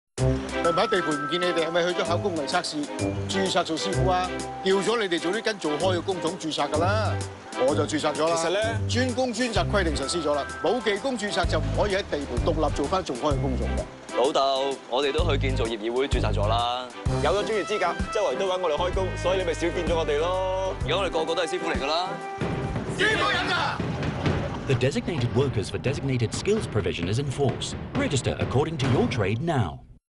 2017 建造業議會「專工專責」規定已實施 廣告(英文台版) - 石修、陳宇琛 [HD]
建造業工人註冊條例「專工專責」規定
The Designated workers for destinated skills provision under the Construction Workers Registration Ordinance is effective now
Registr according to your trade now!
一般建造業項目
建造業機電項目
機械操作項目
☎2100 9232
建造業議會
CONSTRUCTION INDUSTRY COUNCIL